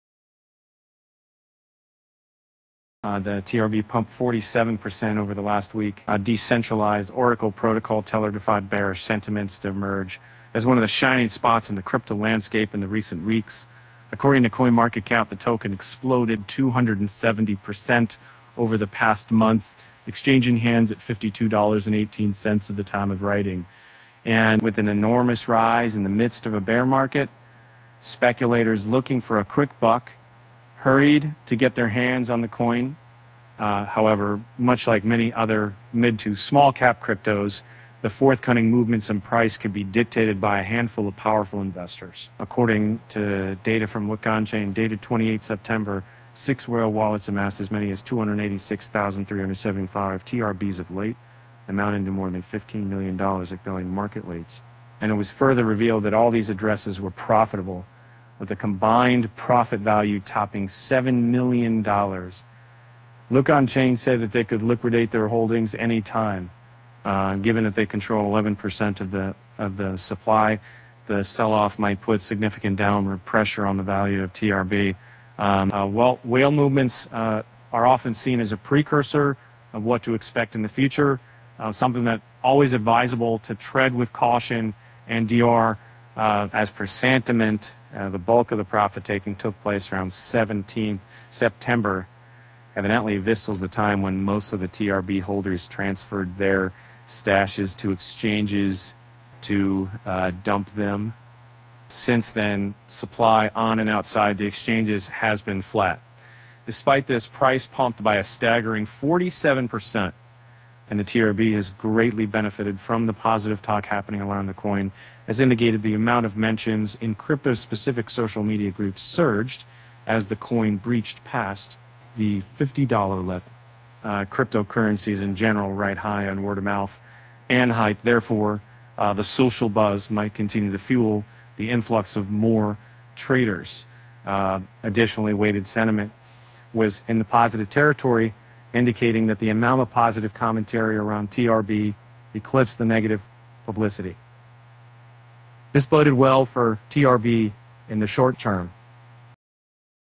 TRB rises 270% as whales pile up: What’s going on?
Decentralized oracle protocol Tellor defied bearish sentiments to emerge as one of the shining spots in the crypto landscape in the recent weeks. According to CoinMarketCap, the token exploded 270% over the past month, exchanging hands at $52.18 at the time of writing.